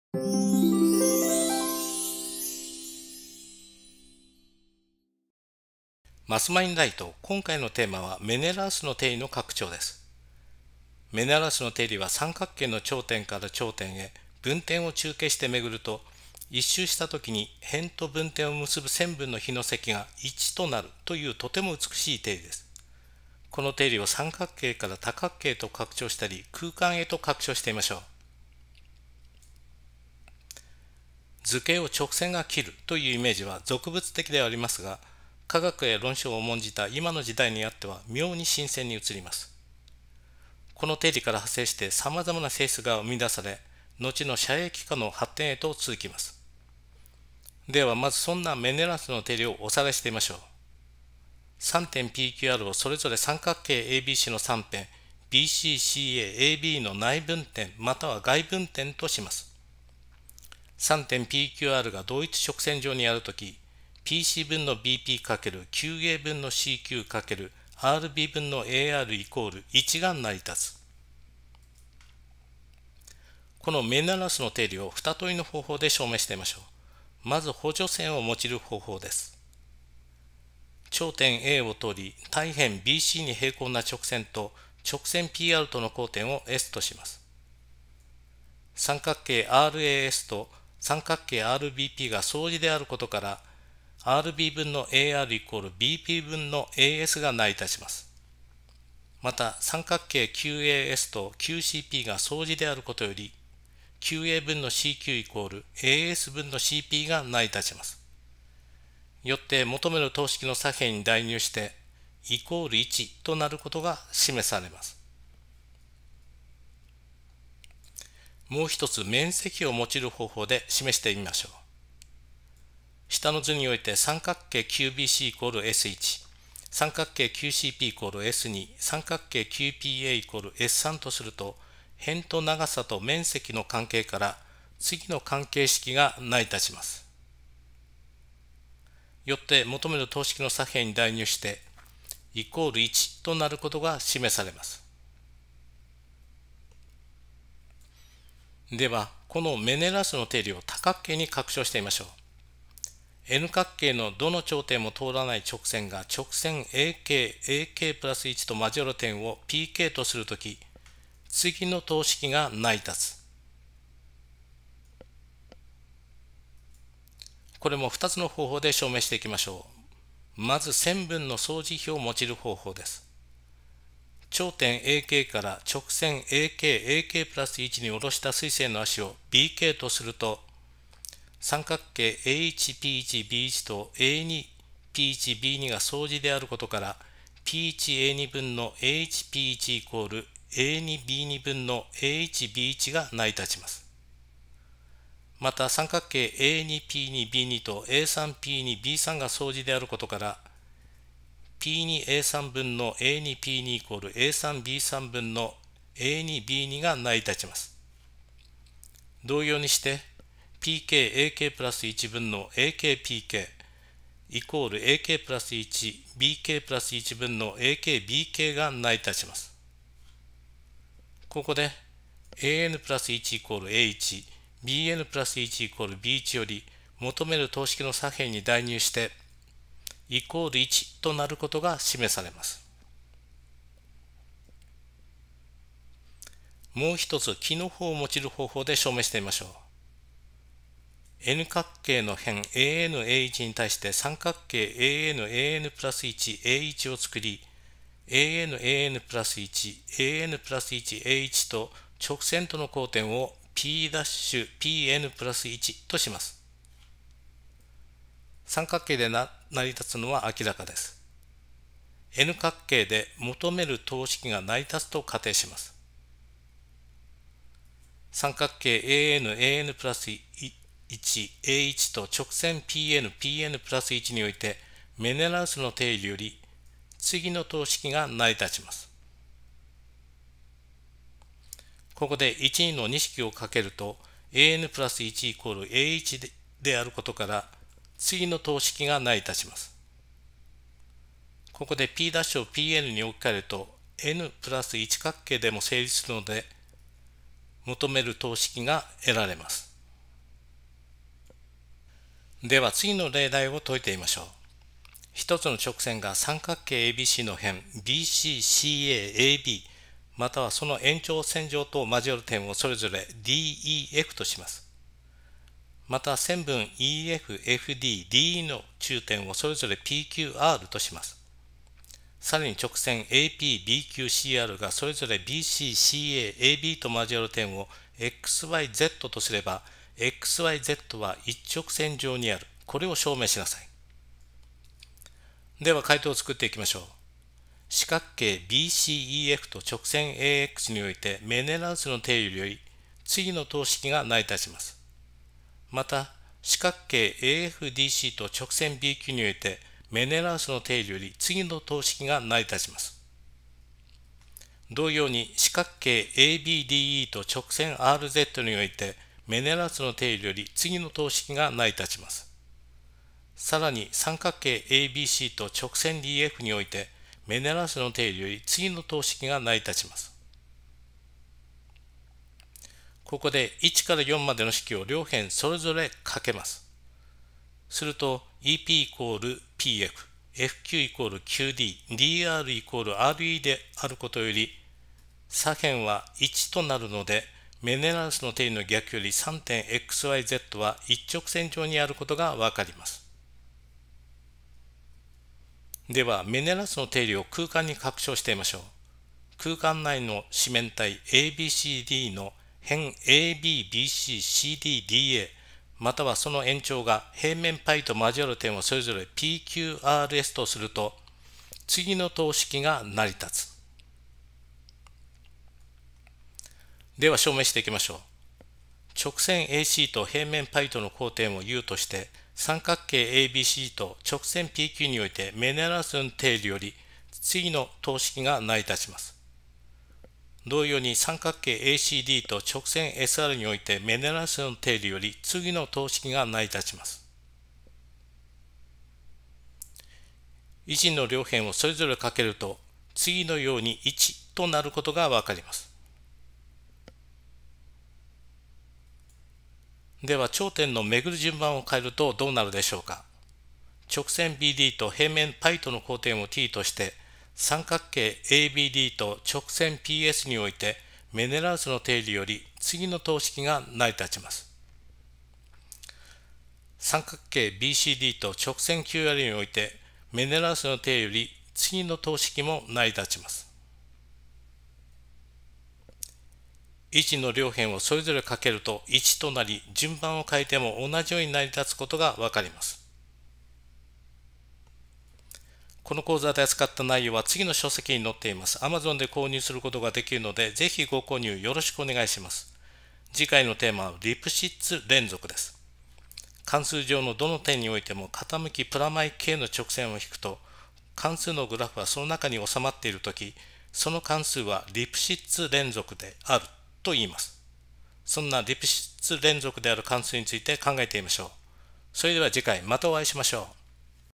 MML第93回 メネラウスの定理の拡張
メネラウスの定理は、三角形の頂点から頂点へ分点を中継して巡ると一周したときに辺と分点を結ぶ線分の比の積が1となる、というとても美しい定理です。この定理を、三角形から多角形へと拡張したり、空間へと拡張してみましょう。